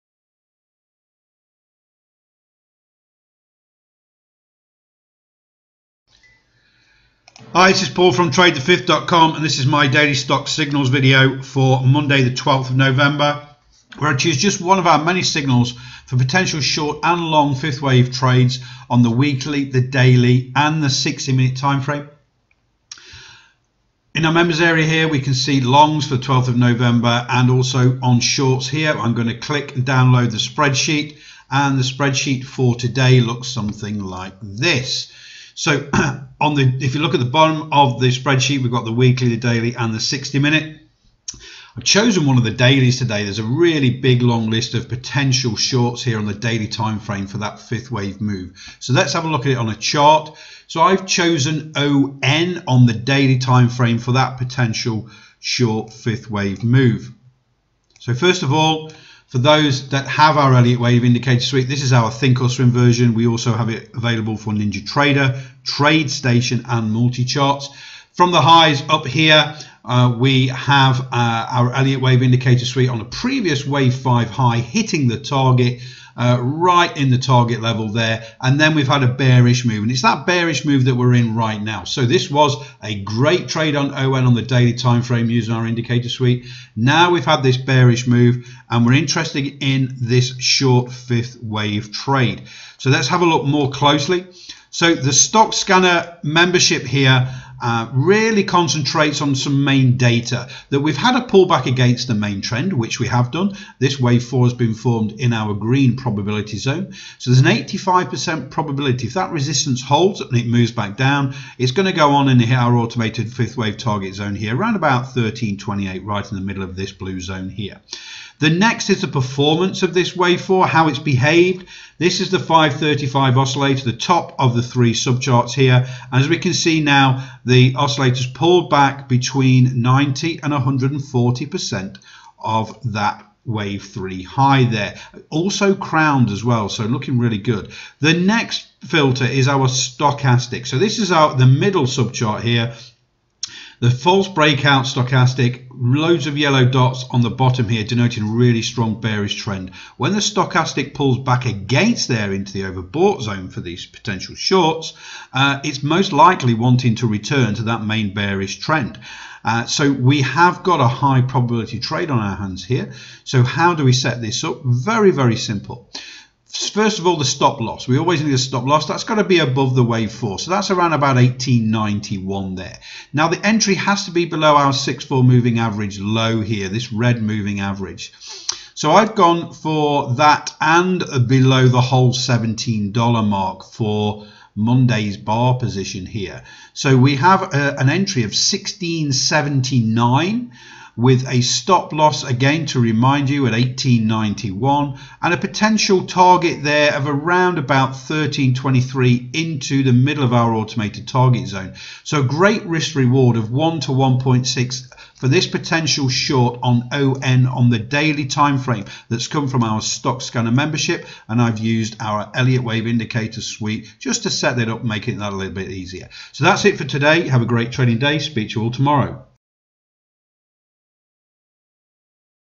TradeTheFifth Stocks Signals Video for potential Short Swing Trad for ON
Watch the Stocks Swing Trading Setup video for a potential Short trade for ON using the Elliott Wave Indicator Suite